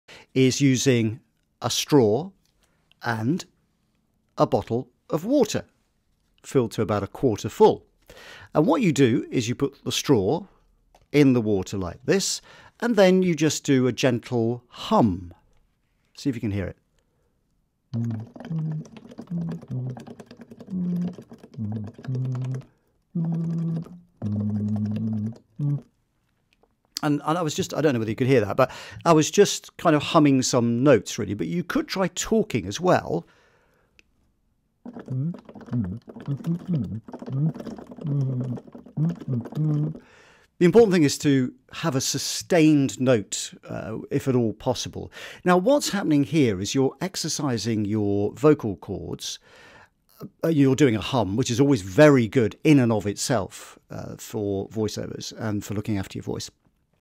Voice Warm Ups with Bottle & Straw
Here is am effective vocal exercise for voice actors and singers.